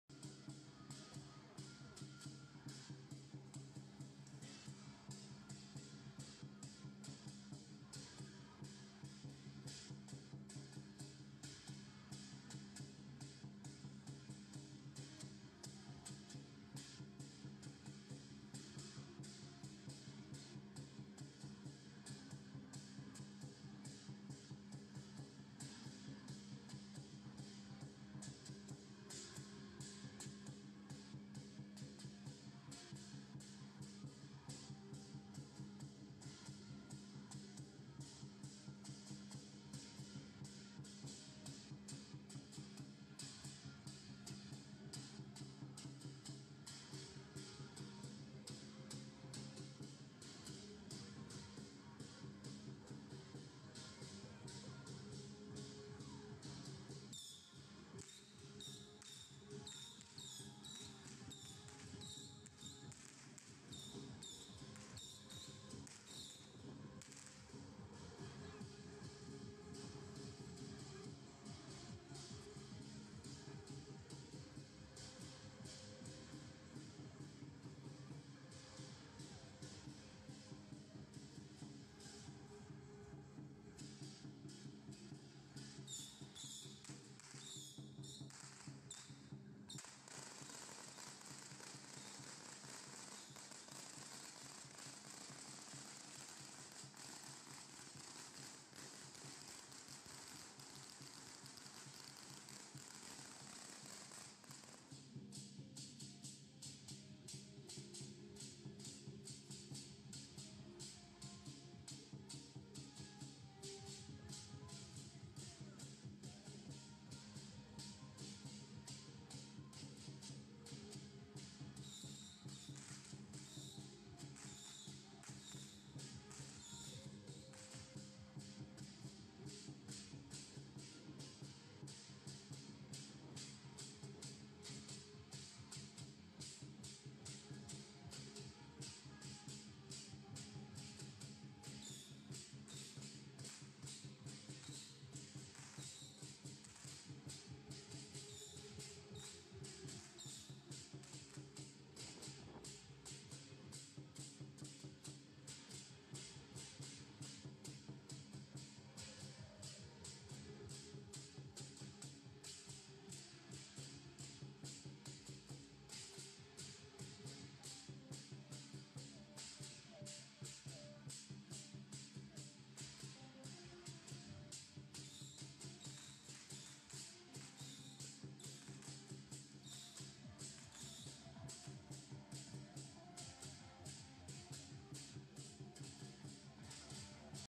隨手拍廟會#1121 漂亮太子乩身 持刺球打先鋒 先入廟參禮 再為主帥轎 鳴炮開路 入廟參禮
偶爾會亂拍些去各景點走走的生活日常 !!

Here to share with you every religious event I photographed